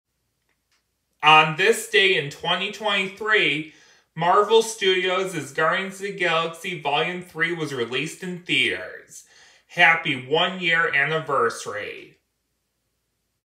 Happy 1 Year Anniversary to Guardians of the Galaxy Vol 3 2023
On this day in 2023, Marvel Studios Guardians of the Galaxy Vol 3 was released in theaters! Happy 1 Year Anniversary! Plus Happy Cinco De Mayo for people who celebrate!

BTW, I saw this 3rd and final movie in the Guardians trilogy in theaters with my cousin for his 25th Birthday last year and I liked it and thought it was emotionally good, even the hallway fight and spaceship rescue scenes!